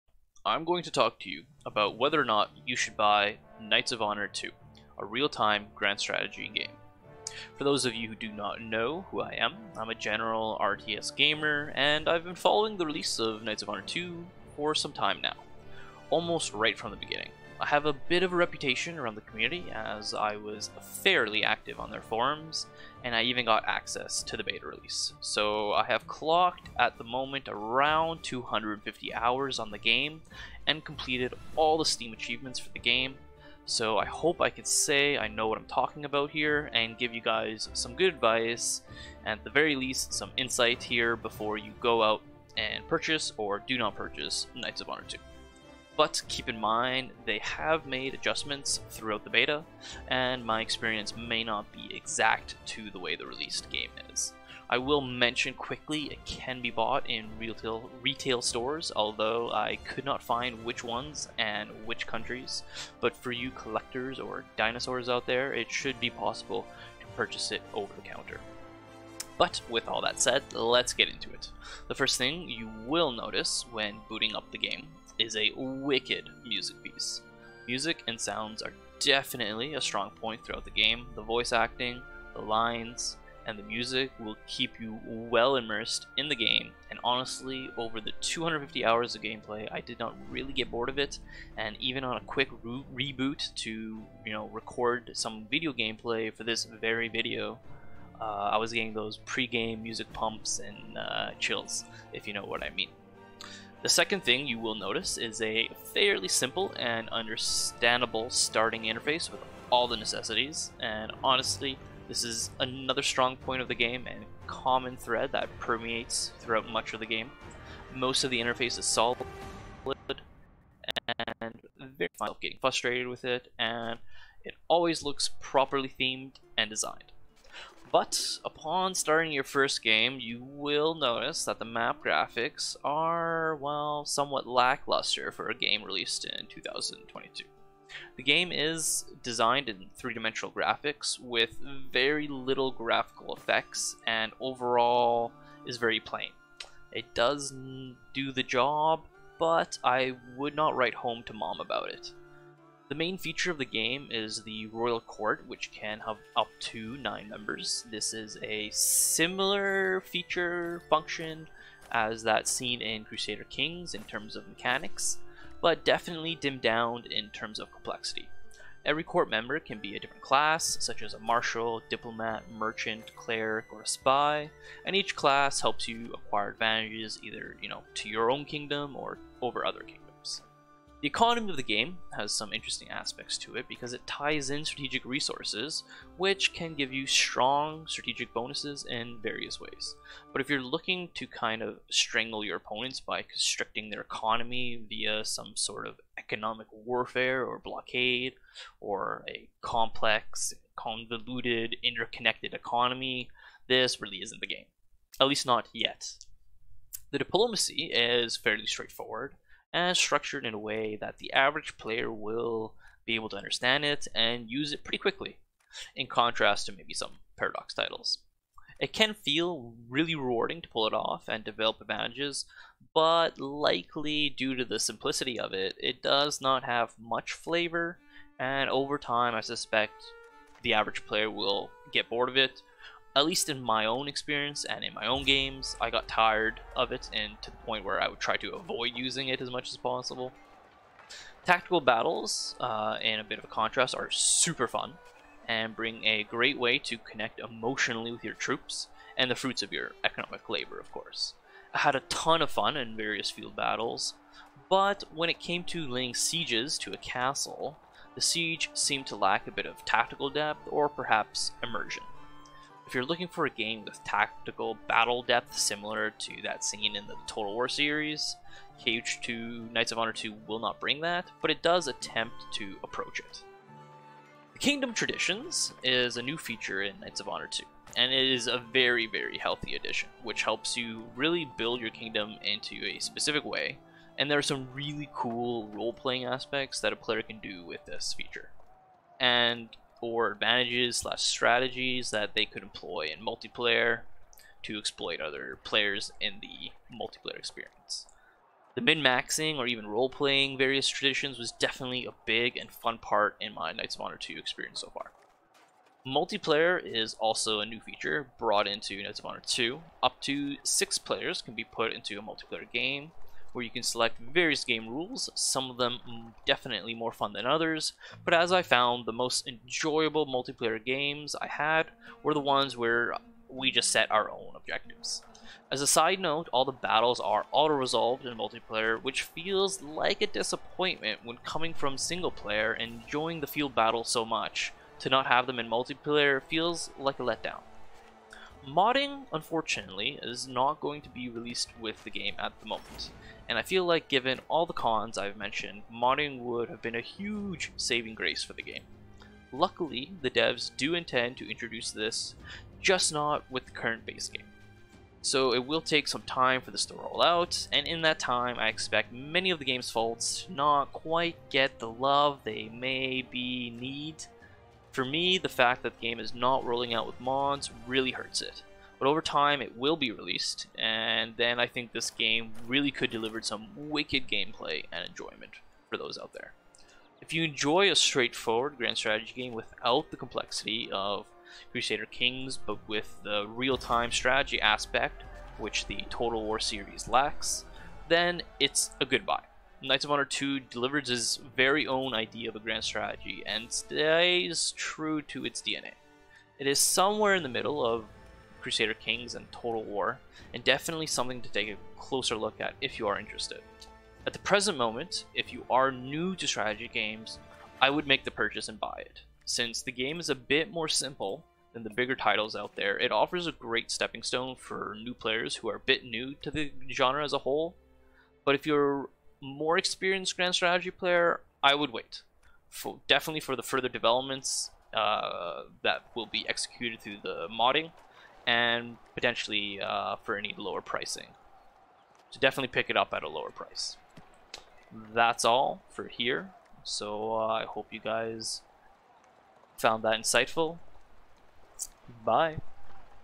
Buy or Not? Knights of Honor 2
So Knights of Honor 2 is coming out, should you buy it or should you not? What is the are the games best and worst aspects?